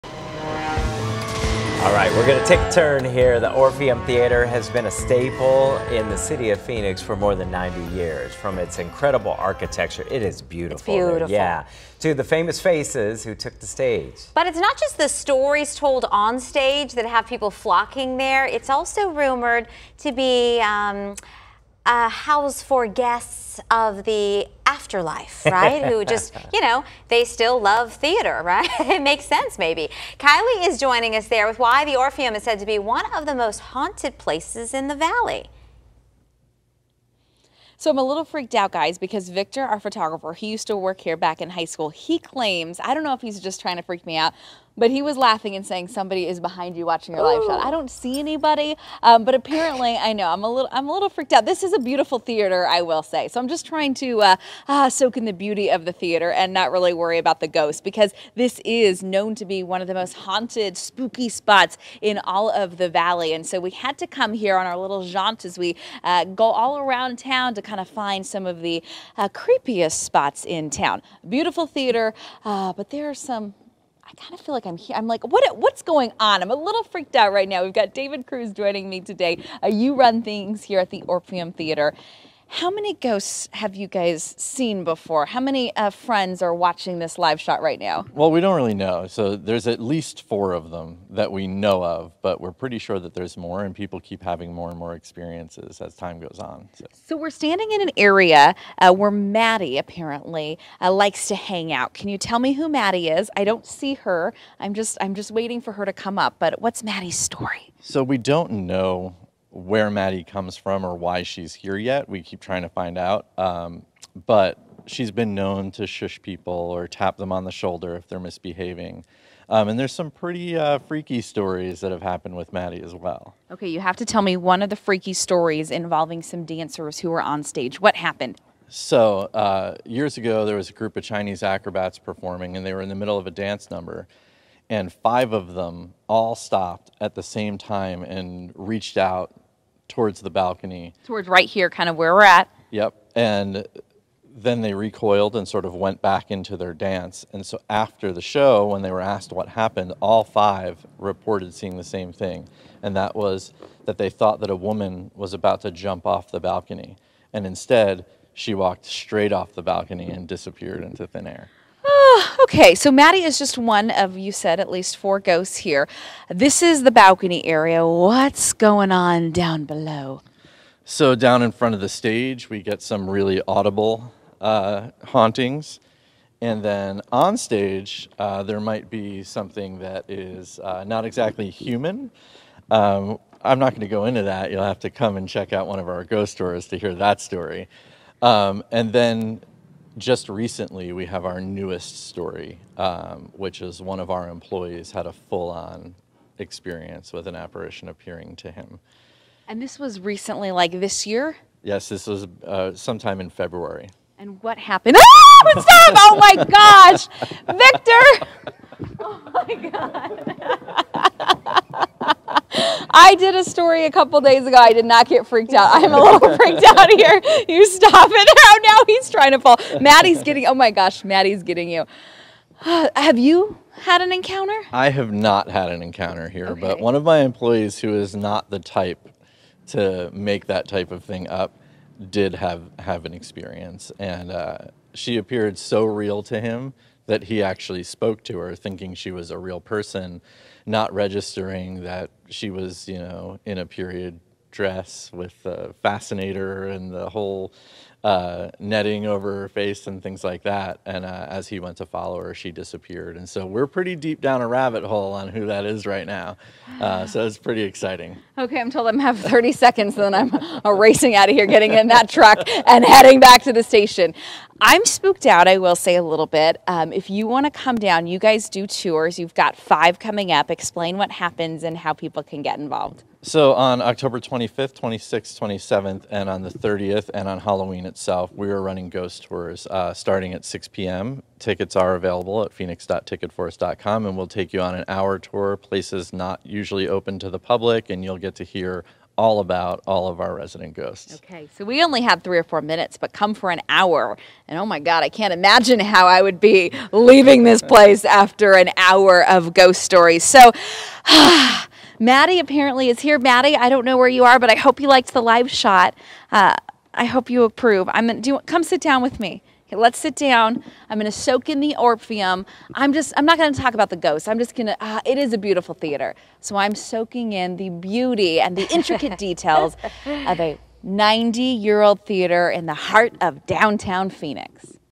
Ghostly guests at the Orpheum Theater in Phoenix
Check out the Orpheum Theater in Phoenix this Halloween where you may see some ghostly guests